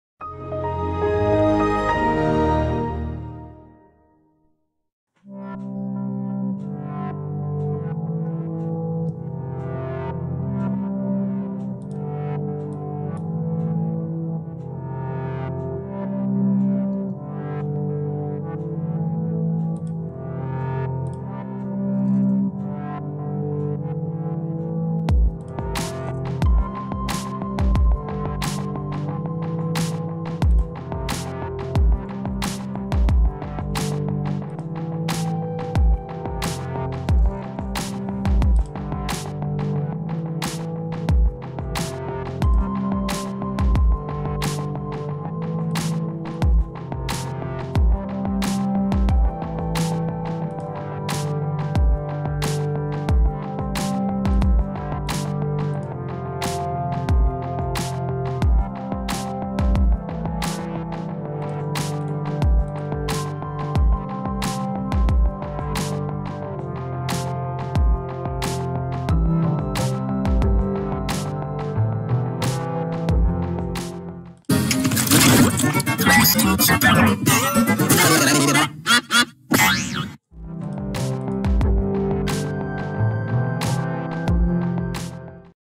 How To Make G-Major 84 On Sony Vegas Pro (Fixed)
Made on ‎‎Monday, ‎April ‎22, ‎2019, ‏‎12:30:35 am to 12:31:42 am

Let's make G-Major 84 on Sony Vegas Pro.

Actual date uploaded while it was unlisted: May 12, 2019.

Updated on August 20, 2020, 2:42 pm.

Fixed because on (0:55 - 1:00) and (1:02 - 1:08), it put Classic, A17, Drums, unpitched (minimum echo), when it was supposed to be elastique, Pro.

Updated Time: 2:42 pm